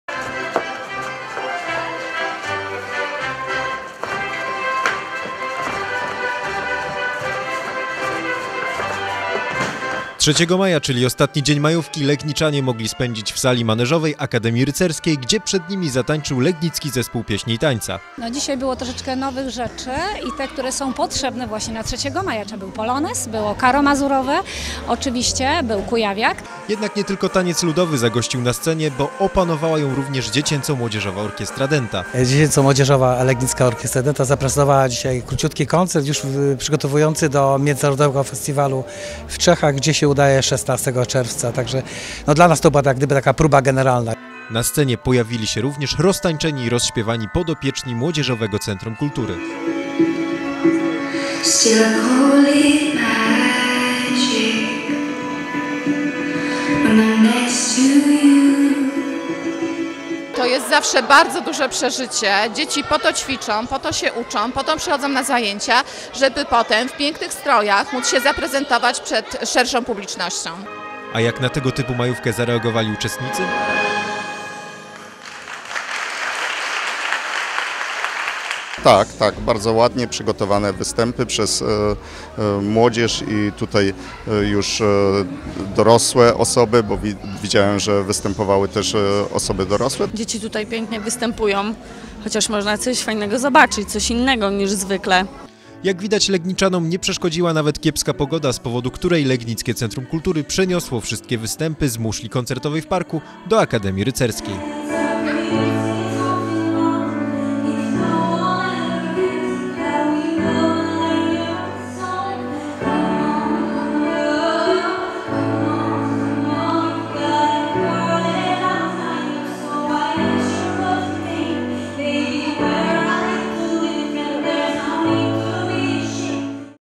3 maja w Akademii Rycerskiej!
Środa 3-go maja była ostatnim dniem weekendu majowego. Z tej okazji Legnickie Centrum Kultury przygotowało dla legniczan szereg artystycznych występów. Na scenie Sali Maneżowej zaprezentowała się m.in. Dziecięco-Młodzieżowa Orkiestra Dęta, legnicki Zespół Pieśni i Tańca oraz podopieczni Młodzieżowego Centrum Kultury.